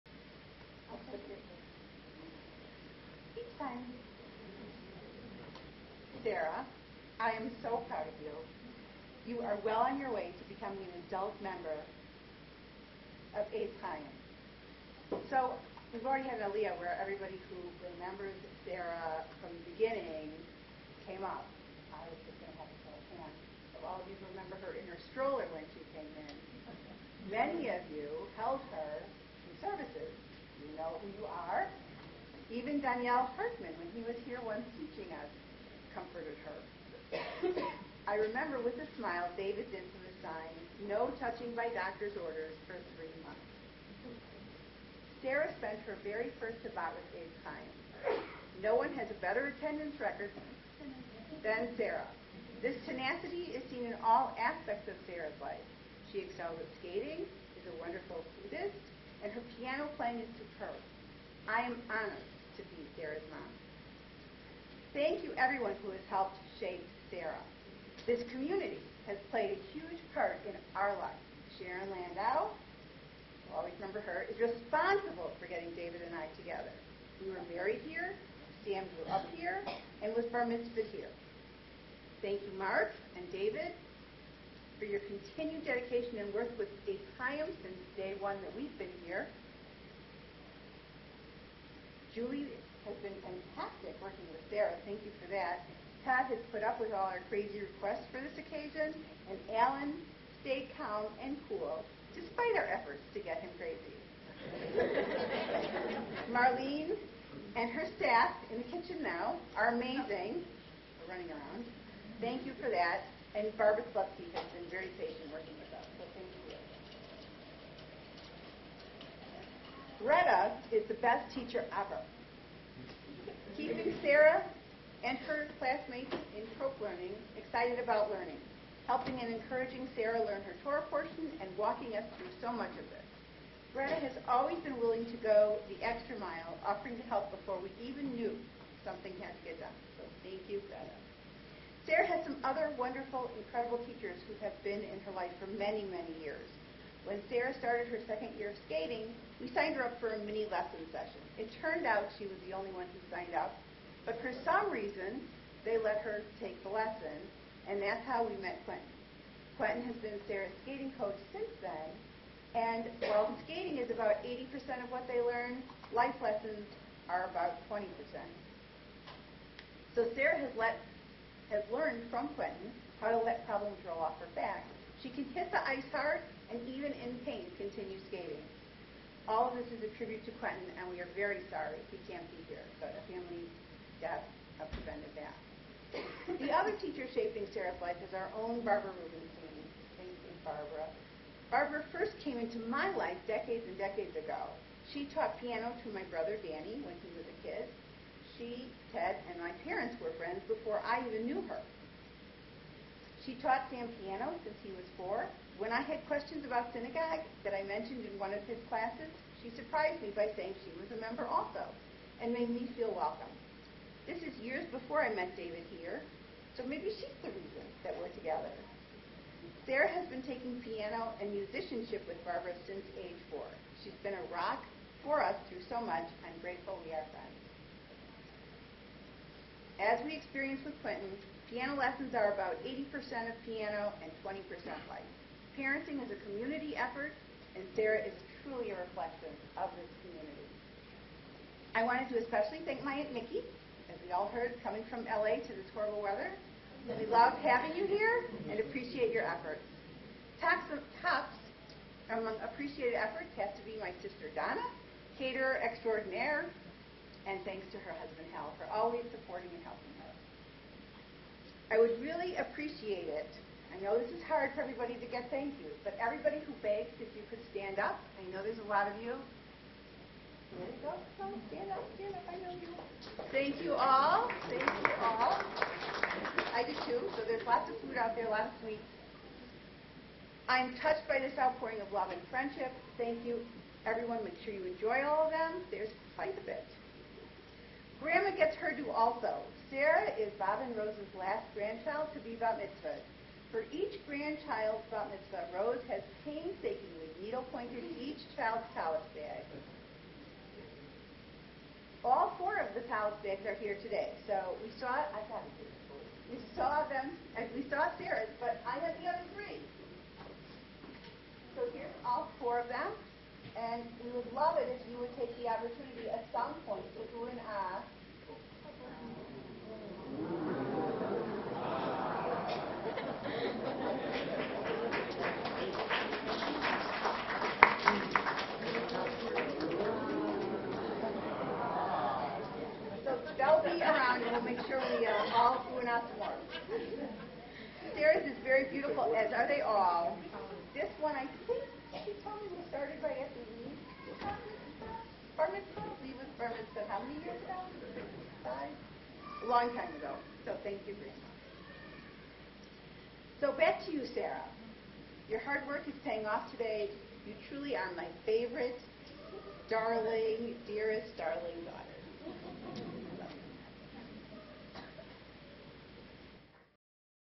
Sarah Aronson Bat Mitzvah Mom's speech
Sarah Aronson Bat Mitzvah Mom's speech.  Aitz Hayim, Highland Park, IL